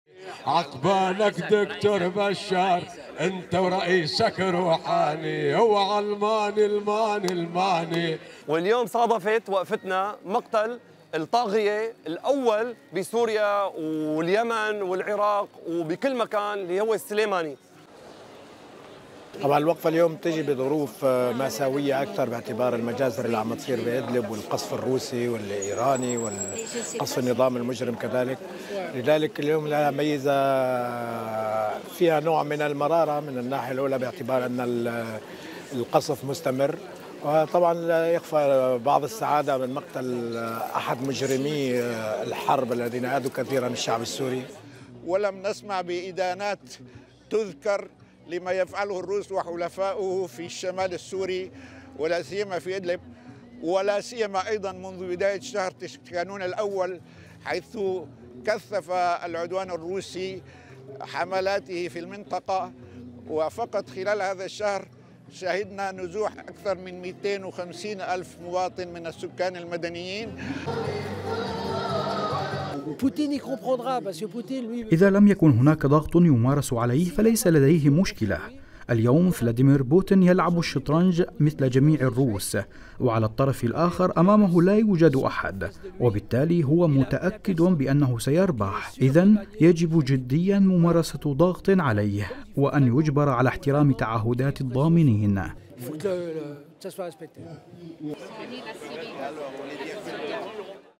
وقفة تضامنية في باريس تنديدا بالمجازر في إدلب
كما نظم السوريون في باريس وقفة تضامنية تتنديدا بالمجازر التي ترتكبها ميليشيا أسد والاحتلال الروسي  بحق المدنيين في إدلب مطالبين المجتمع الدولي بالتحرك السريع للحد من هذه الجرائم...

- محمد جزار
UAE: